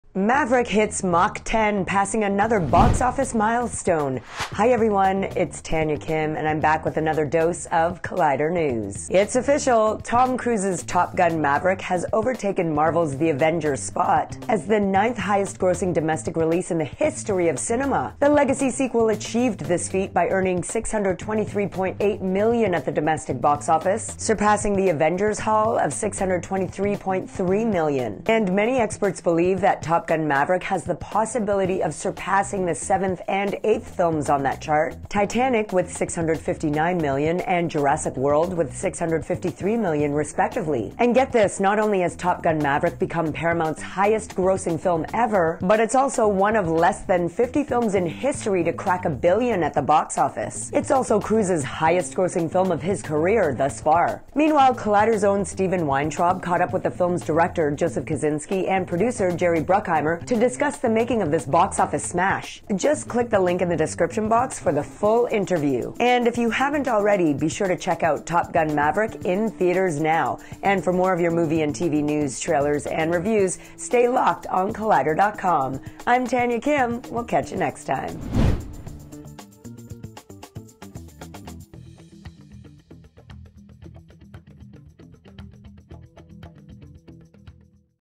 Top Gun: Maverick Hits Mach 10 As Ninth-Highest Domestic Grossing Film in History
At this point, we’re starting to wonder when Top Gun: Maverick will finally cease crossing milestones as it continues to dominate the summer box office ahead of even highly-anticipated Marvel titles. Unlike its titular hero, Maverick has hit Mach 10 and just keeps going, having passed yet another box office milestone and outdoing not just this year’s Thor: Love and Thunder, but the superhero team-up that started it all. The high-flying Tom Cruise film has officially surpassed The Avengers at the domestic box office, stealing the Marvel film’s spot to become the ninth-highest grossing domestic release in cinema history. Take that, Iron Man!

To achieve such a spot in cinematic history, the legacy sequel brought in $623.8 million at the domestic box office, surpassing The Avengers’ haul ($623.3 million) by about half a million dollars. With the end of its theatrical release window still nowhere in sight, many experts believe that the film can come out of the cinematic dogfight even more victorious, with the possibility to surpass the films that sit in seventh and eighth place on the chart — Titanic with $659 million, and Jurassic World with $653 million, respectively.